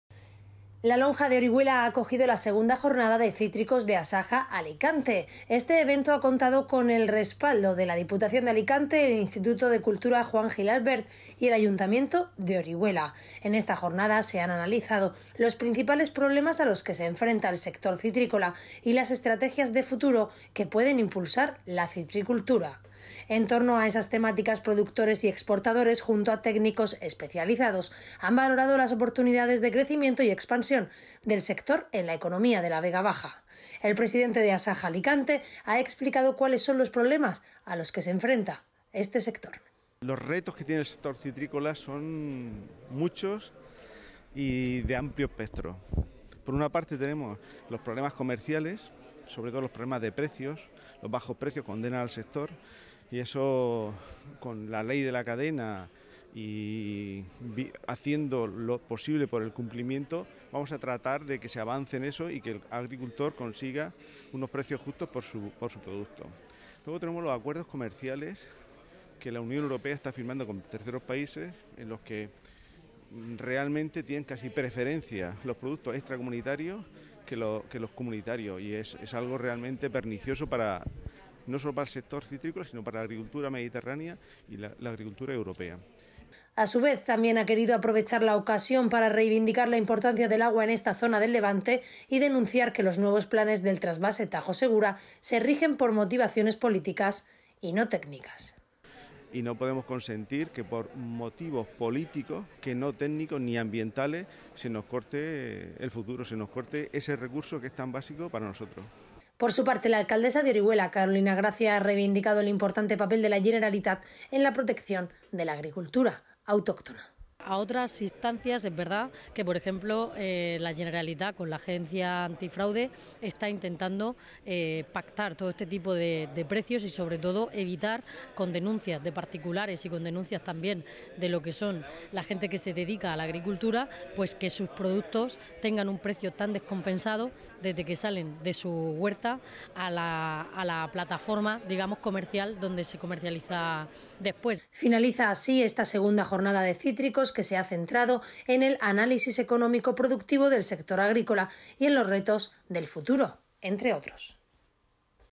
ASAJA Alicante organiza la II Jornada de Cítricos
La Lonja de Orihuela ha acogido la segunda Jornada de Cítricos de ASAJA Alicante. Este evento ha contado con el respaldo de  la Diputación de Alicante, el Instituto de Cultura Juan Gil-Albert y el Ayuntamiento de Orihuela.

En esta jornada se analizarán los principales problemas a los que se enfrenta el sector citrícola y las estrategias de futuro que pueden impulsar la citricultura.  En torno a esas temáticas, productores y exportadores junto a técnicos especializados, han valorado las oportunidades de crecimiento y expansión del sector en la economía de la Vega Baja.

El presidente de Asaja Alicante ha explicado cuáles son los problemas a los que se enfrenta este sector.

A su vez, también ha querido aprovechar la ocasión para reivindicar la importancia del agua en esta zona del levante y denunciar que los nuevos planes del Trasvase Tajo Segura se rigen por motivaciones políticas y no técnicas.

Por su parte, la alcaldesa de Orihuela, Carolina Gracia, ha reivindicado el importante papel de la Generalitat en la protección de la agricultura autóctona.

Finaliza así esta segunda jornada de Cítricos, que se ha centrado en el análisis económico productivo del sector agrícola y en los retos del futuro entre otros.